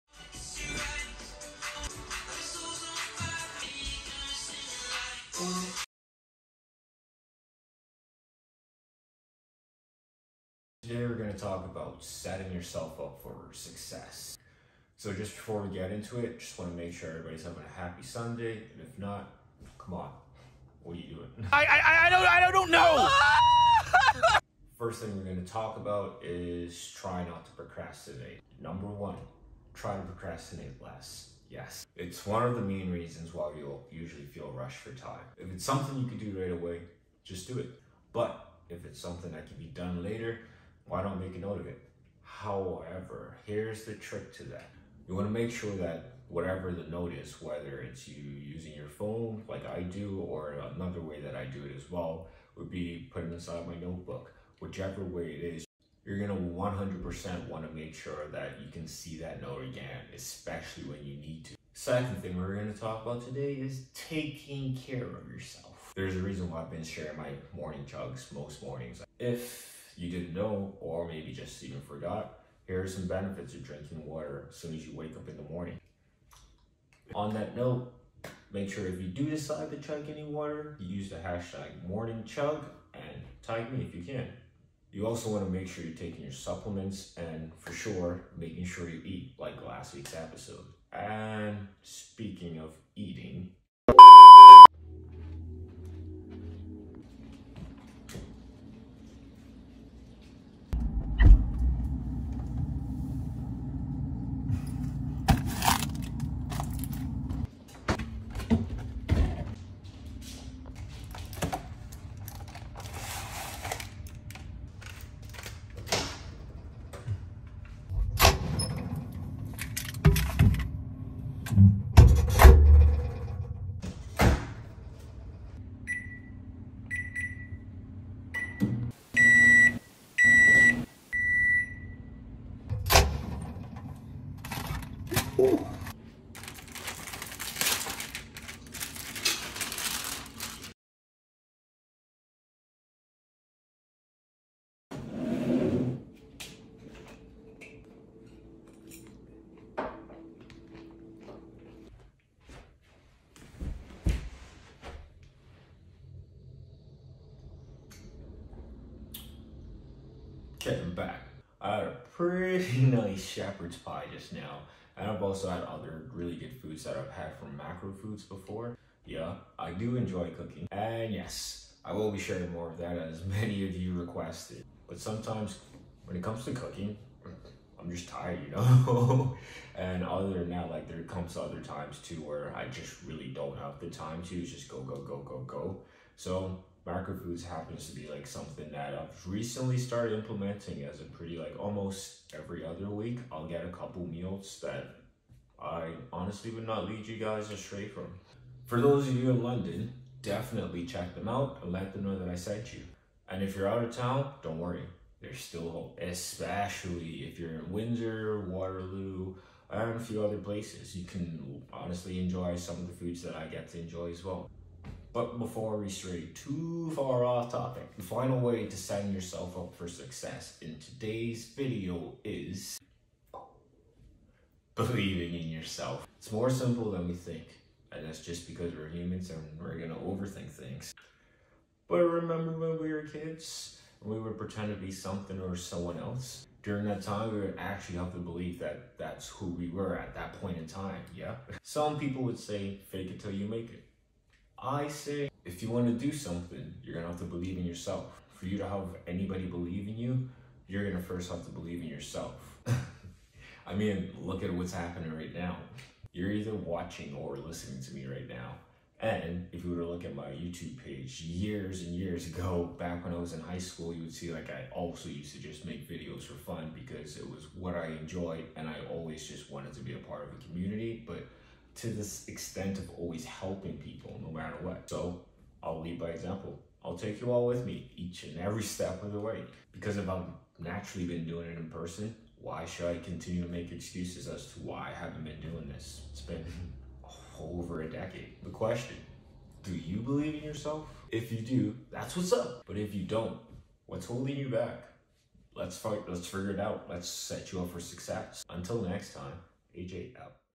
Setting Yourself Up For Success | Sunday Vlog | AyJay Gibson
Happy Sunday!

As promised here are ways for setting yourself up for success!
Remember to rest as well and think good thoughts

What's something that you feel holds you back in your daily life?